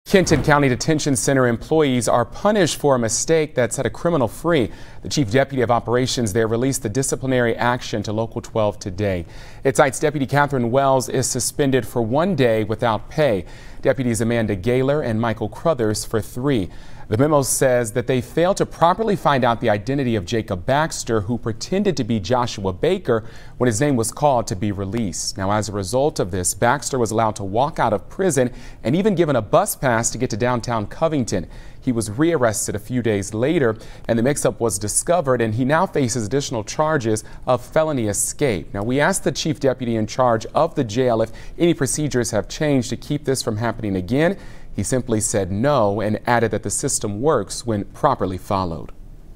3 Kenton County deputies suspended after inmate mistakenly released
KENTON COUNTY, Ky. (WKRC) - Three Kenton County Detention Center employees have been punished for a mistake that set a suspect free.

The chief deputy of operations released the disciplinary action to Local 12 Friday. It cites Deputy Katherine Wells is suspended for one day without pay. Deputies Amanda Gaylor and Michael Crouthers are suspended for three days.

The memo says they failed to properly find out the identity of the Jacob Baxter, who pretended to be Joshua Baker when his name was called to be released.

As a result, Baxter was allowed to walk out of prison and was even given a cab voucher to get to downtown Covington. He was re-arrested a few days after the mixup was discovered and faces an additional charge of felony escape.

Local 12 asked the chief deputy in charge of the jail if any procedures have changed to keep this from happening again. He said no and that the system works when properly followed.